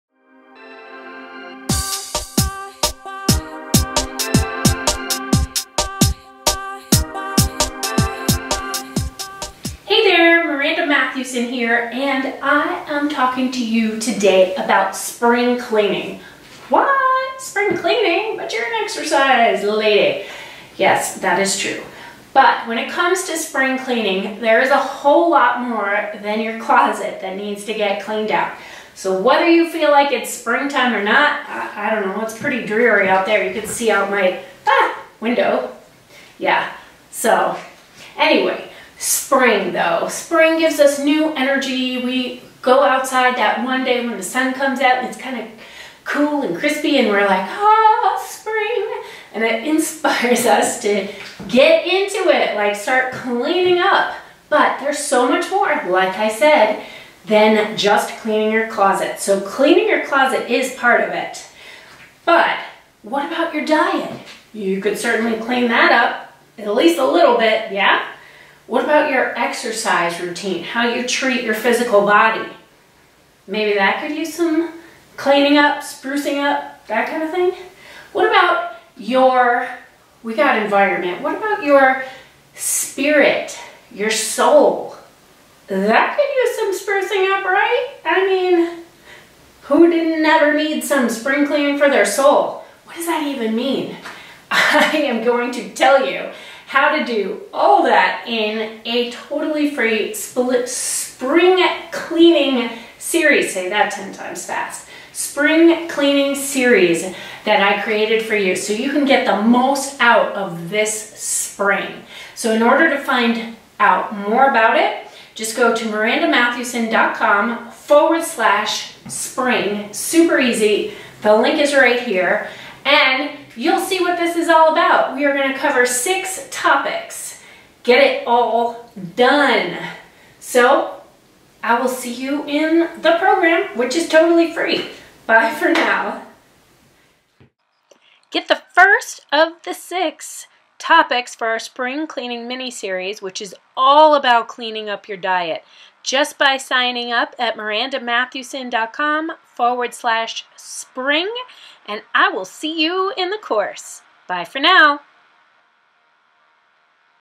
Spring Cleaning for Healthy Living free mini course
Join the Spring Cleaning for Healthy Living free mini course

This free mini course teaches healthy living tips for beginners and veterans as well.  We'll be focusing on 6 topics that will teach you how clean up your diet, exercise, stress management, sleep habits, environment, and soul. Basically how to improve yourself overall and  improve self confidence at the same time.